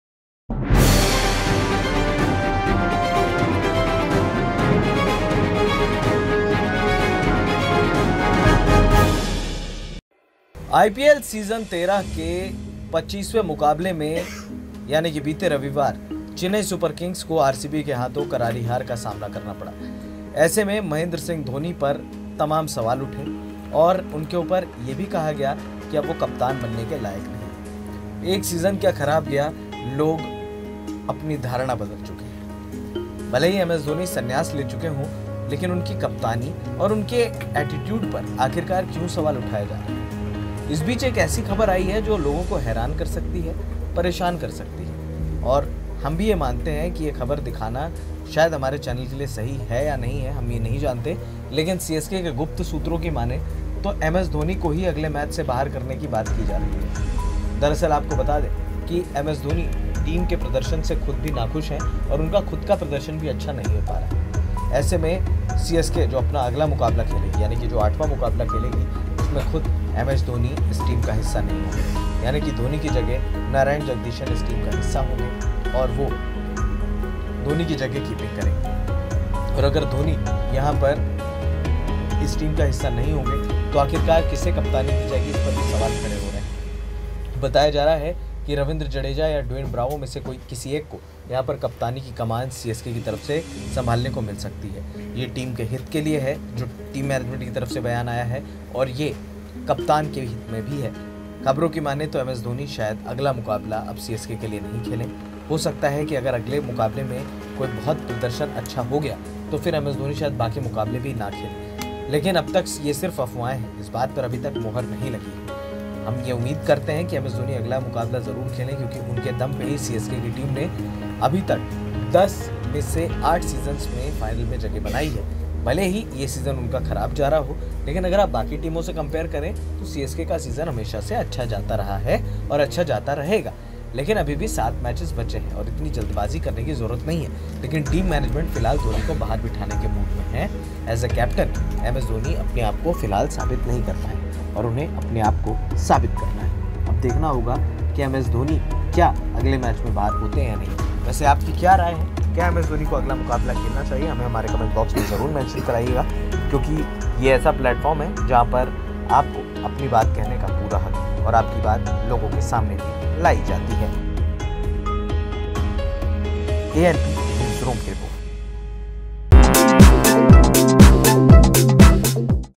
RCB से मिली हार के बाद धोनी ने खुद को किया CSK की टीम से बाहर!मज़बूरी में CSK ने रैना को वापस बुलाया
इस ख़ास रिपोर्ट में देखें RCB  से मिली हार के बाद धोनी ने खुद को किया CSK की टीम से बाहर!मज़बूरी में CSK ने रैना को वापस बुलाया

ANPNEWS
anpnews